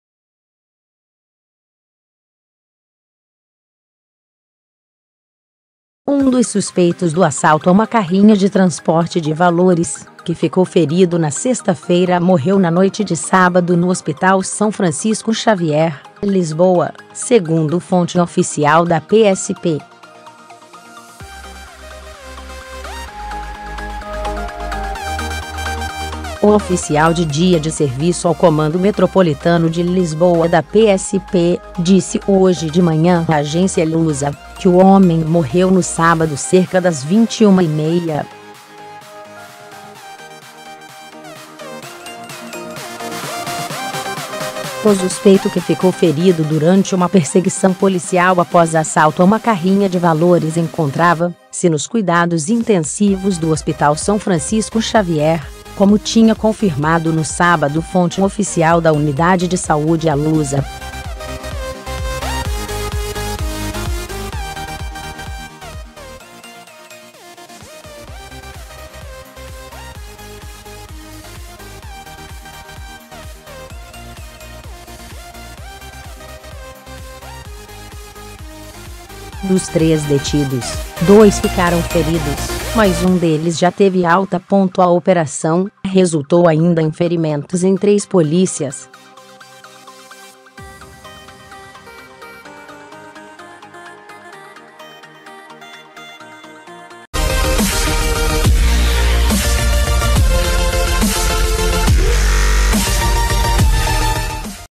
Morreu o suspeito de assalto a carrinha de valores que estava no hospital
Um dos suspeitos do assalto a uma carrinha de transporte de valores que ficou ferido na sexta-feira morreu na noite de sábado no Hospital São Francisco Xavier, Lisboa, segundo fonte oficial da PSP.

O oficial de dia de serviço ao Comando Metropolitano de Lisboa da PSP disse hoje de manhã à agência Lusa que o homem morreu no sábado cerca das 21:30.

O suspeito que ficou ferido durante uma perseguição policial após assalto a uma carrinha de valores encontrava-se nos cuidados intensivos do Hospital São Francisco Xavier, como tinha confirmado no sábado fonte oficial da unidade de saúde à Lusa.

Este homem que morreu no sábado é um dos três suspeitos detidos durante a perseguição policial após assalto a uma carrinha de valores na sexta-feira em Lisboa. Dos três detidos, dois ficaram feridos, mas um deles já teve alta. A operação resultou ainda em ferimentos em três polícias.